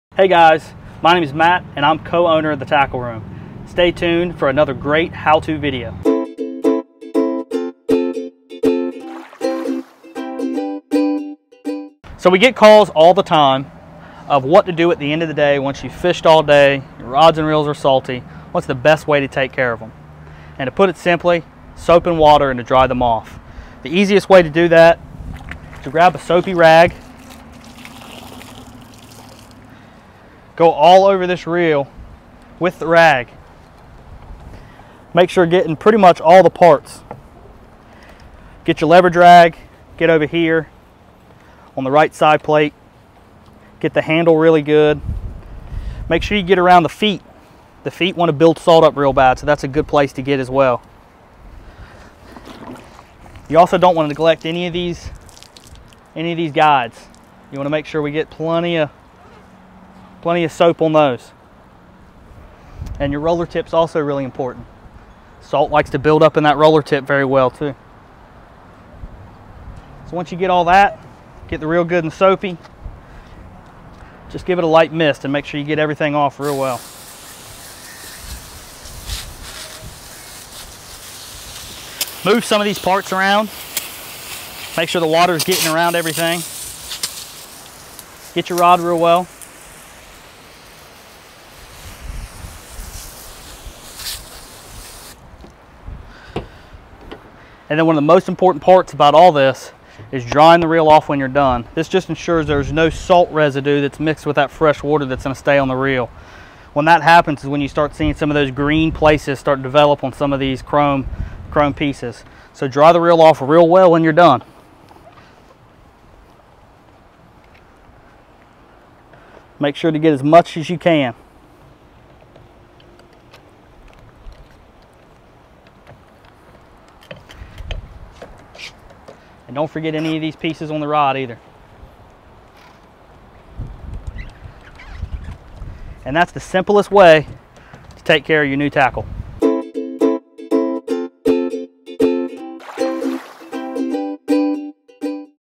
How to Clean a Fishing Reel
How to clean a fishing reel after a day of offshore fishing.  Saltwater builds up and leads to harmful corrosion.  Follow our easy tips for taking care of your saltwater rods and reels.  Shop for rods, reels, lures and more where orders $50+ ship free every day.